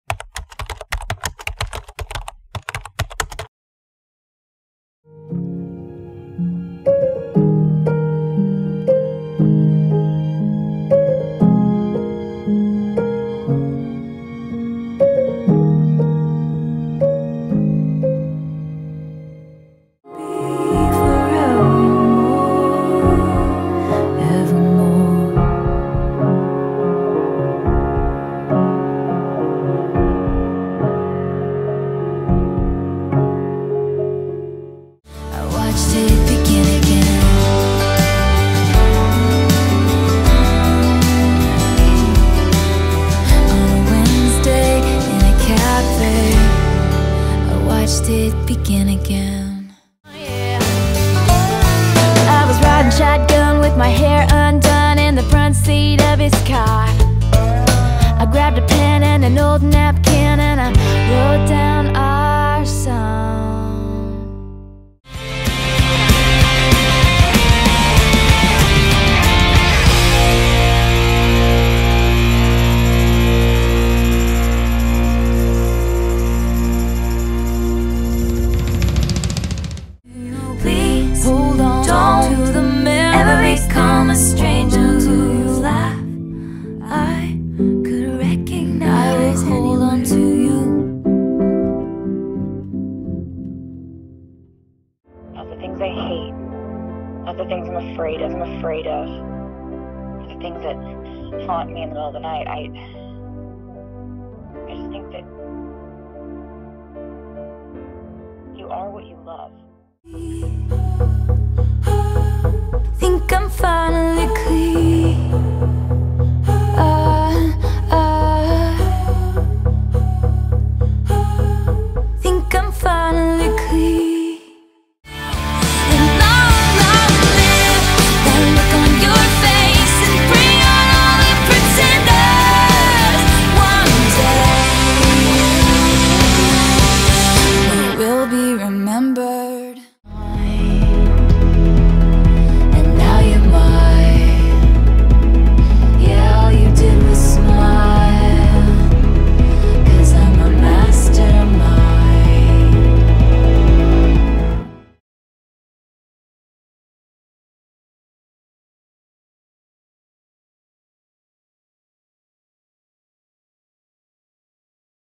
Ranking Taylor Swift's album closers
These are my favorite album closers, based on how well they close the album in my opinion. What's your favorite album closer?